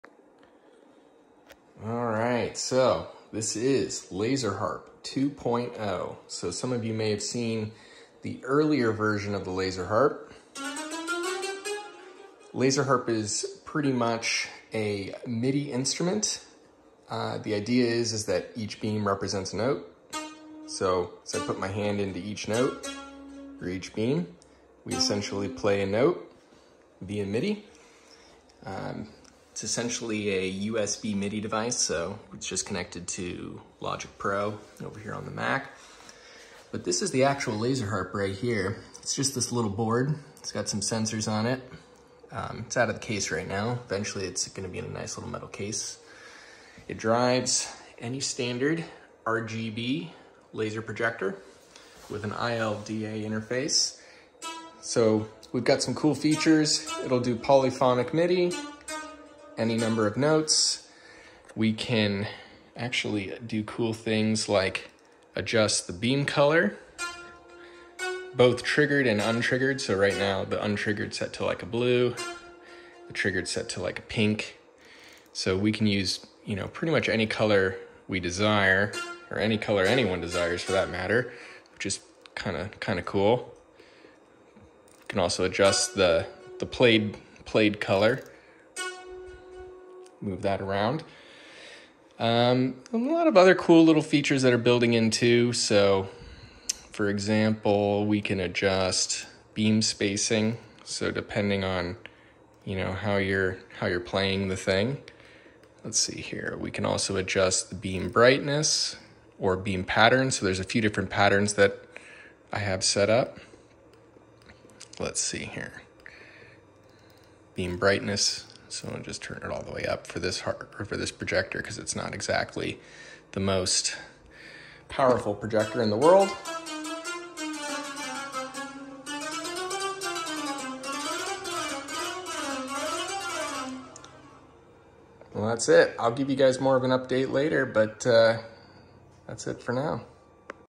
Laser Harp 2.0 Demo
Working on next generation laser harp. This is a quick demo of its operation.